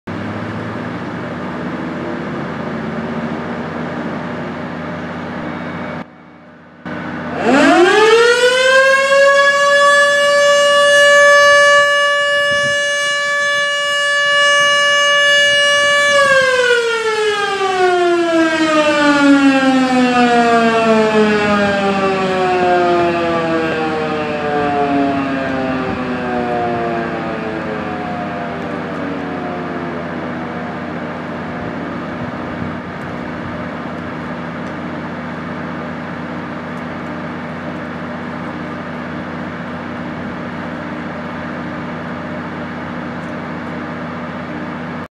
St.Jacobs, Ontario Fire Dept Federal Signal Model 2 Siren Test 12/9/19
So I went to St. Jacobs, Ontario to see this Model 2 for testing at 19:00 hours.

Siren: Federal Signal Model 2
Location: St.Jacobs, Ontario, Woolwich Township St.Jacobs Fire Station.

Testing: Monday's at 6:57pm or 7pm

Today's Testing: 19:01 hours (7:01 pm)

Siren videos, pictures, and more!